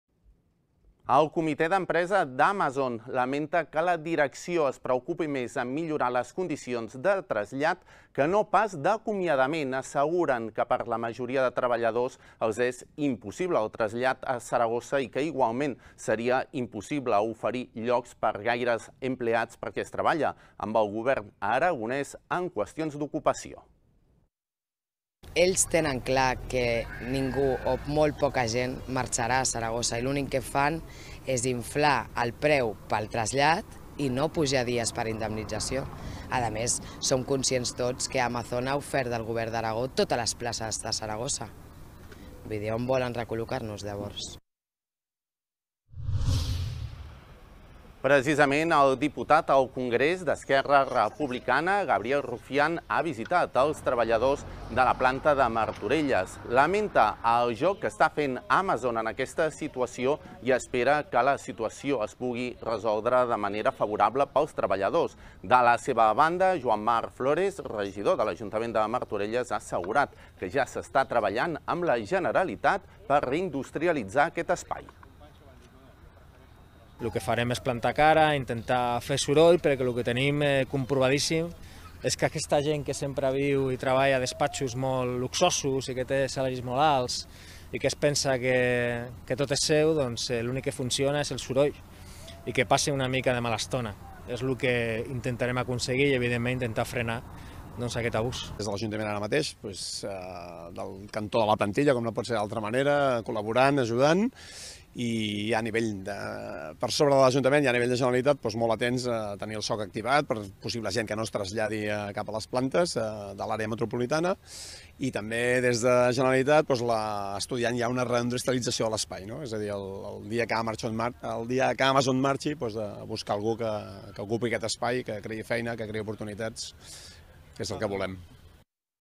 El Diputat al Congrés d'ERC, Gabriel Rufián, dona suport als treballadors d'Amazon a Martorelles
El comitè d'empresa d'Amazon lamenta que l'empresa es preocupi més en millorar les condicions de trasllat que no pas d'acomiadament. Asseguren que per a la majoria de treballadors els és impossible el trasllat a Saragossa i que, igualment, seria impossible oferir lloc per a gaires perquè es treballa amb el govern aragonès en qüestions d'ocupació. Des del comitè esperen un canvi de direcció en allò que està oferint el gegant nord-americà als treballadors de la planta de Martorelles.

Precisament, el Diputat al Congrés d'Esquerra Republicana, Gabriel Rufián, ha visitat als treballadors de la planta de Martorelles. Lamenta el joc brut que fa Amazon en aquesta situació i espera que la situació es pugui resoldre de manera favorable pels treballadors. Per la seva banda, Joan Marc Flores, regidor d'Economia de Martorelles ha assegurat que ja s'està treballant amb la generalitat per reindustrialitzar aquest espai.